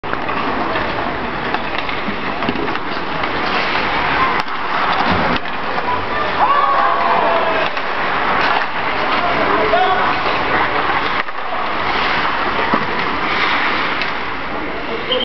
Olofströms IKs första träning 2010/2011
Litet klipp från OIK:s första träning för säsongen.